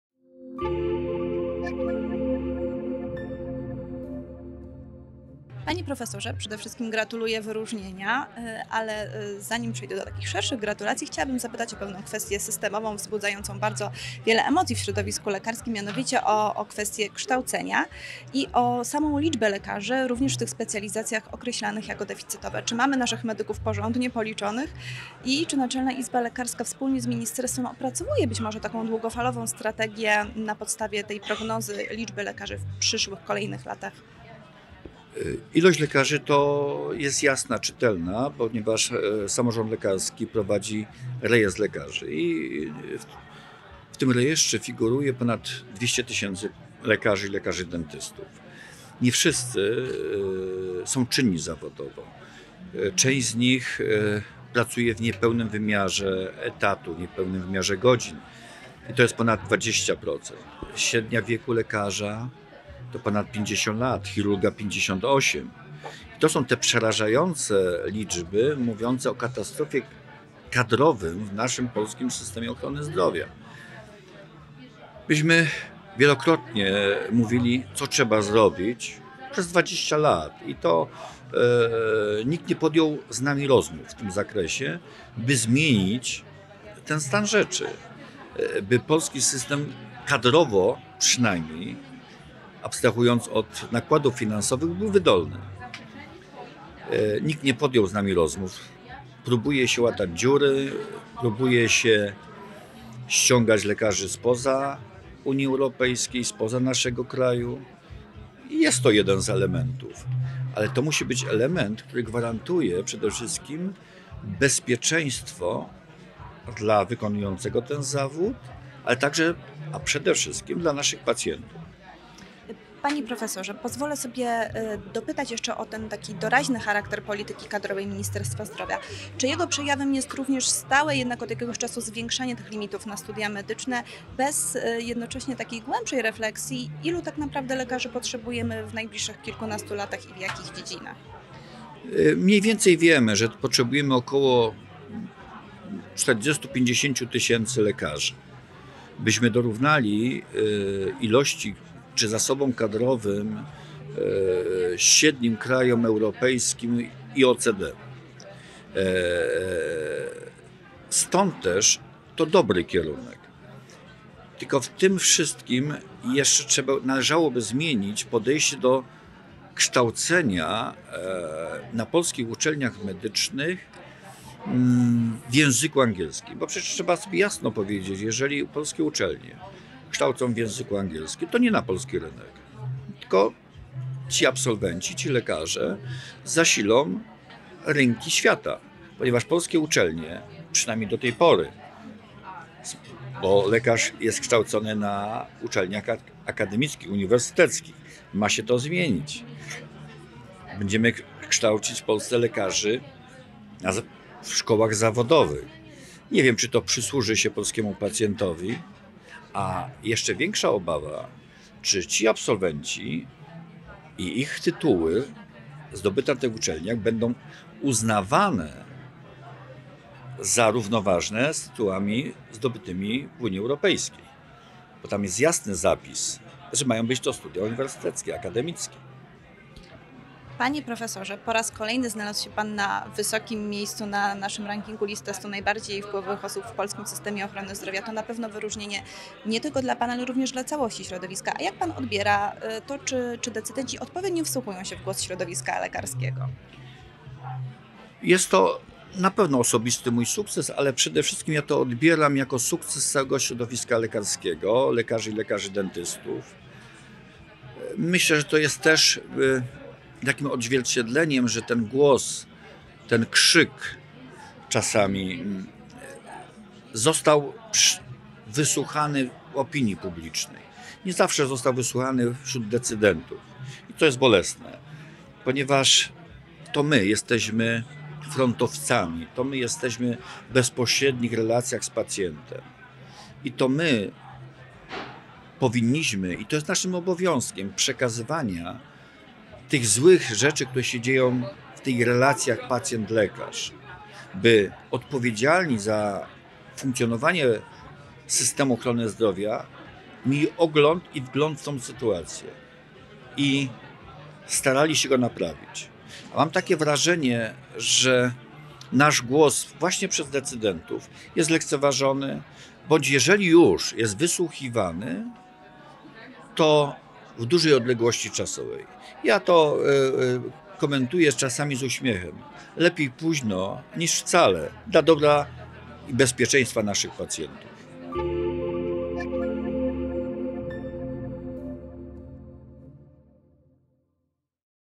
Prof. Matyja o polityce kadrowej MZ: to łatanie dziur | Puls Medycyny
Próbuje się w tej chwili łatać dziury, ściągać lekarzy spoza obszaru Unii Europejskiej. Może to być jeden z elementów polityki kadrowej, ale nie można przy tym zapominać o gwarancji bezpieczeństwa dla polskich pacjentów oraz personelu medycznego - komentuje prof. Andrzej Matyja sytuację kadrową w środowisku lekarskim.